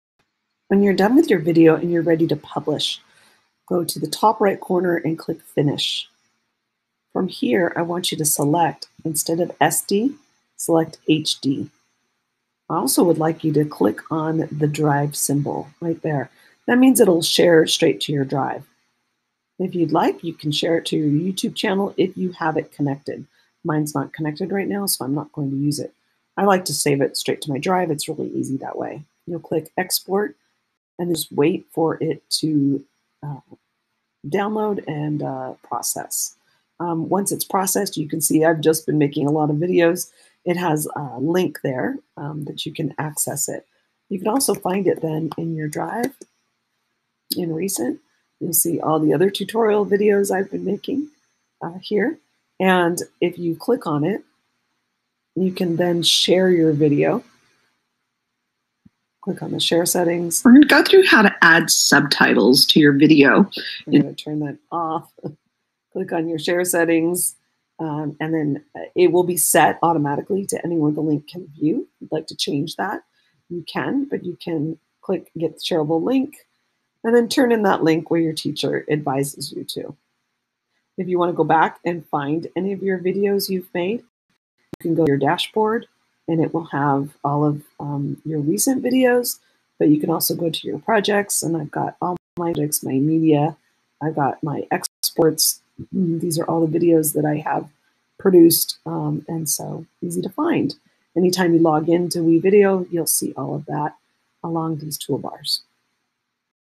How to publish your video using WeVideo
When you finish editing in WeVideo, this is how you publish it for others to see.